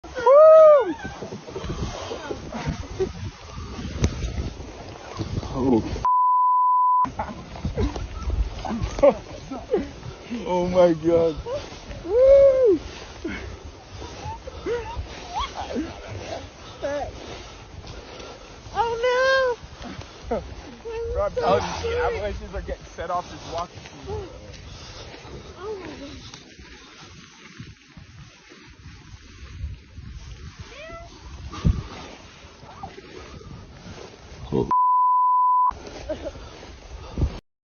Truckee, California ❄
Truckee, California getting dumped on with snow.